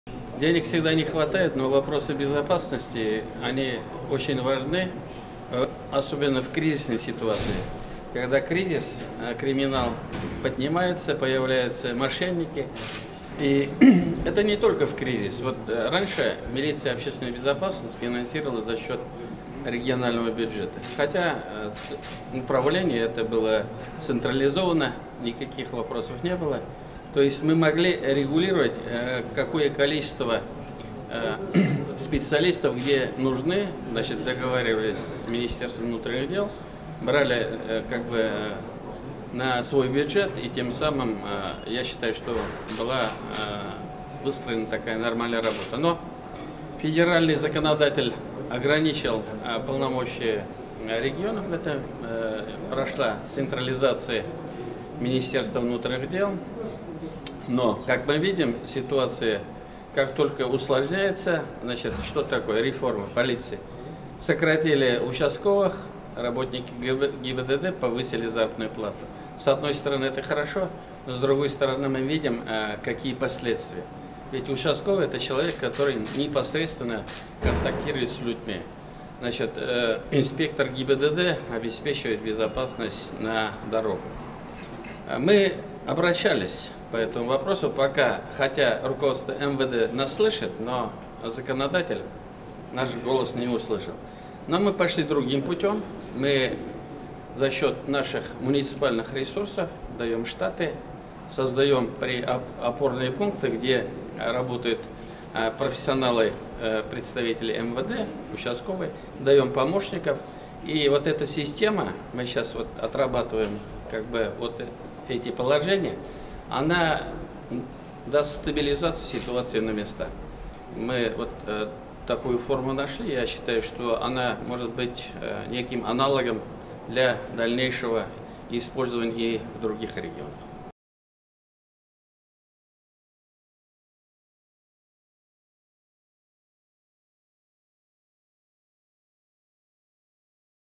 Рустам Минниханов рассказал, как власти помогут полиции бороться с преступностью в кризис
Полиция в ряде районов «проспала» зарождение новых ОПГ, десятки татарстанцев воевали в Сирии на стороне боевиков, а футбольные фанаты сбиваются в ультраправые группировки. Об этом сегодня на коллегии МВД рассказал глава ведомства Артем Хохорин. Выслушавший его Рустам Минниханов сделал особый акцент на борьбу с экстремистами, которые, по его словам, все чаще используют «цветные технологии» и забил тревогу по поводу роста подростковой преступности.